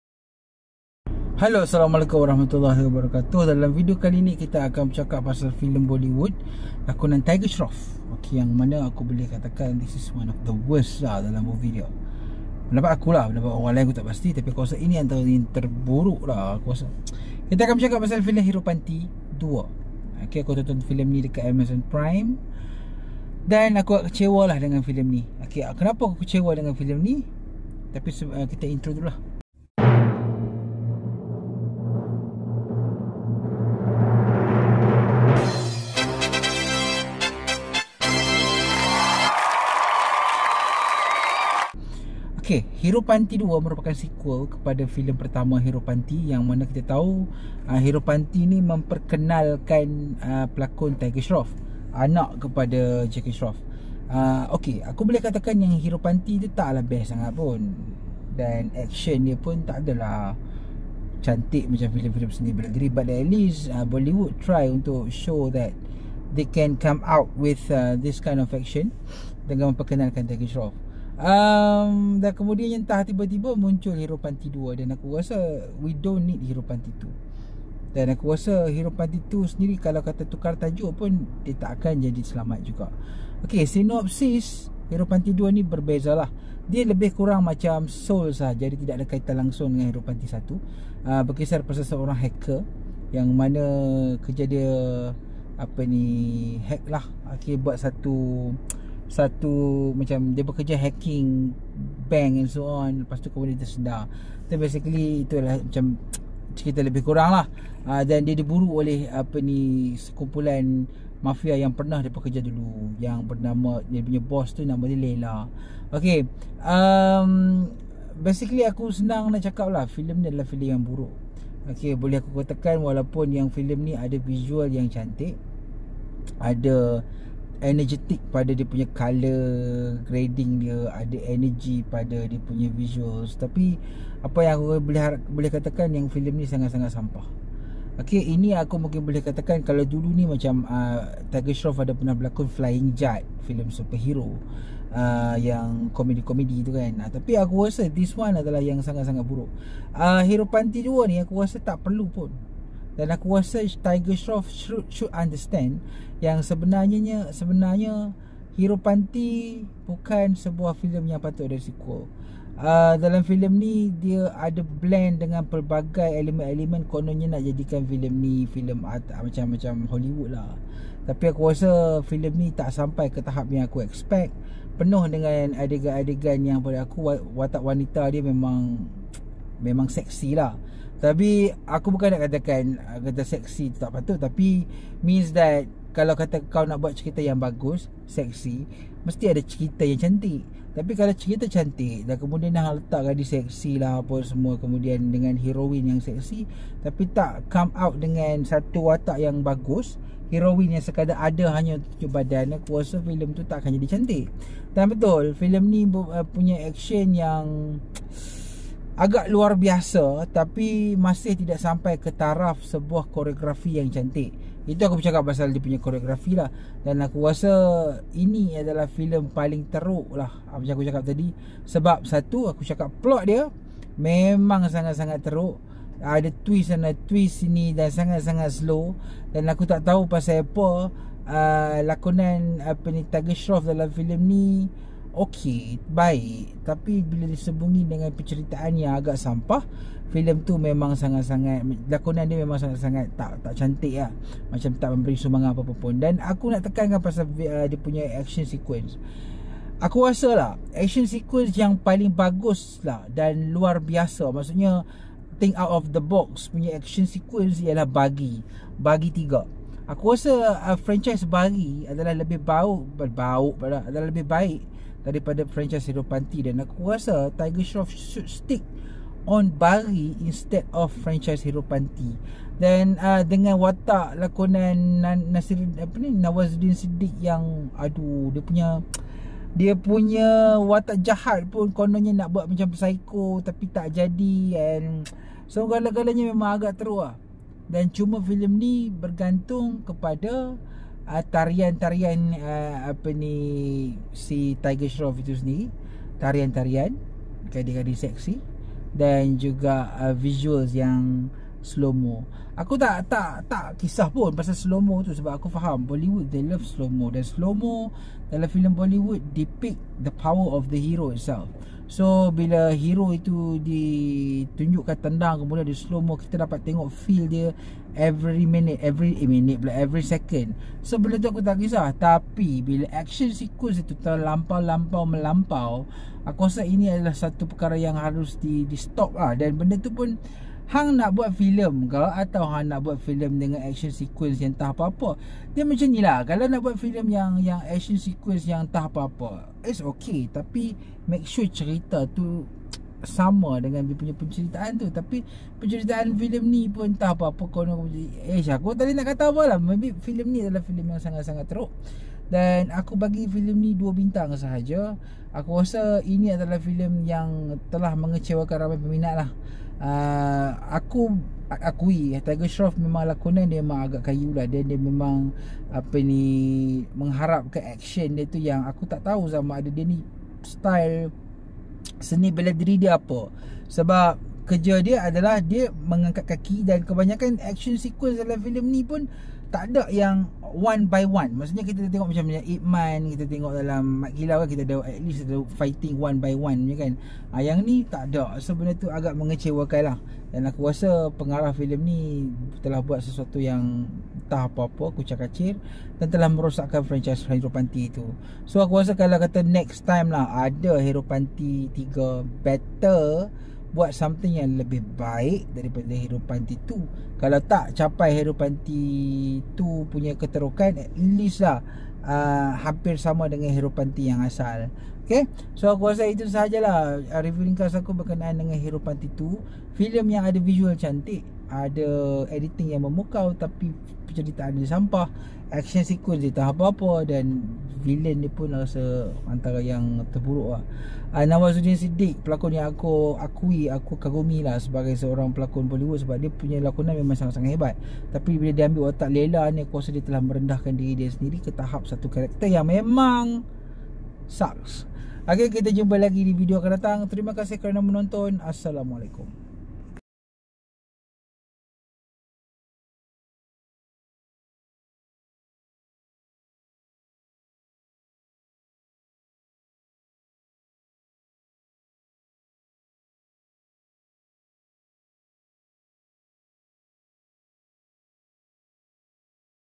Ulasan Filem Heropanti 2 (2022) [SPOILER ALERT]
Heropanti 2 (2022)

Babloo Ranawat, a genius hacker, is tasked with tracking down Laila, the mastermind behind the world's largest cyber-crime yet to occur. Will Babloo be able to stop him?